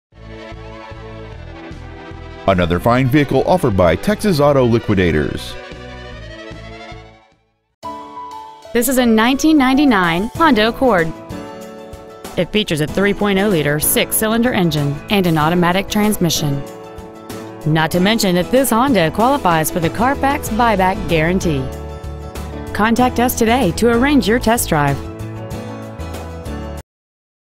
1999 Honda Accord Coupe Arlington TX
We are proud to present this 1999 Honda Accord Coupe .

 Year : 1999

 Make : Honda

 Model : Accord Coupe

 Engine : 3.0L SOHC VTEC V6 engine

 Trans . : Automatic

 Exterior : Green

 Miles : 111,660

 Interior : Tan

 7445 US Hwy 287

 1999 Honda Accord Coupe Arlington TX